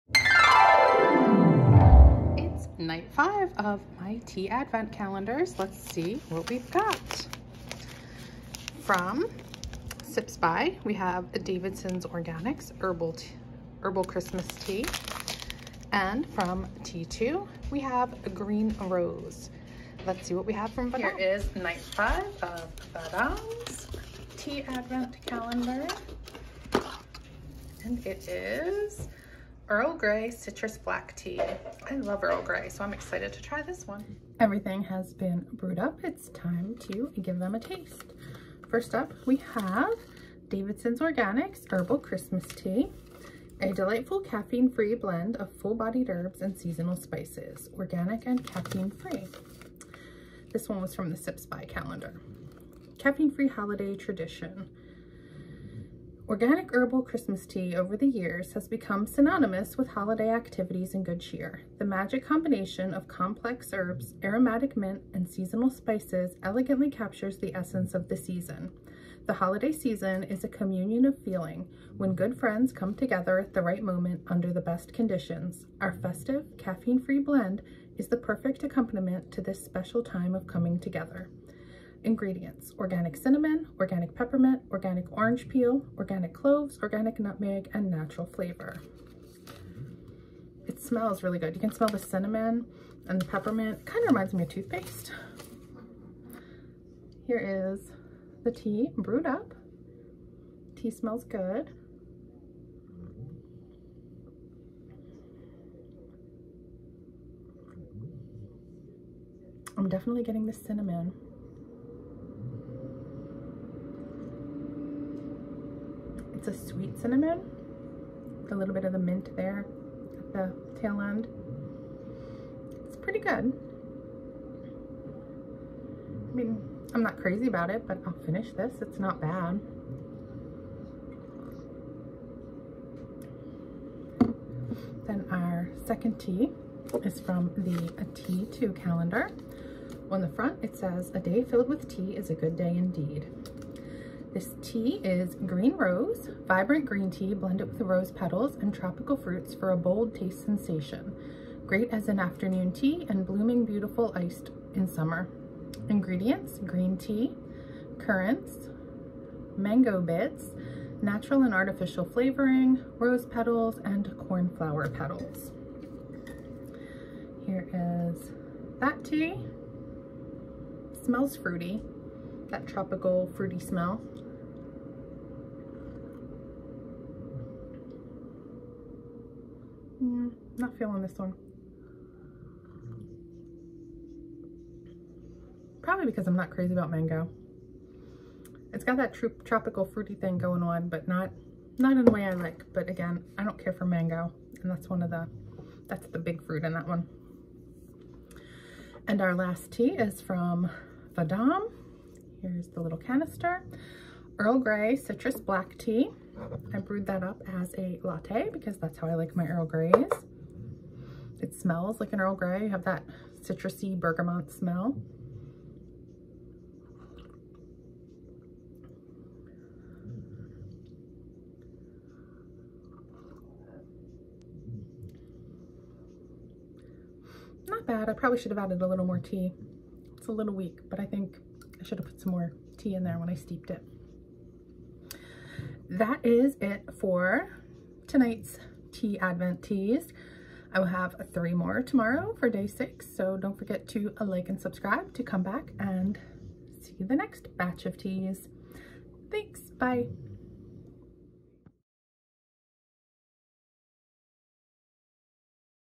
Tea Advent Day 5
Opening and trying the teas from my tea advent calendars. Day 3 for
sips By, Vahdam and T2.

SipsBy Get $5 off your first box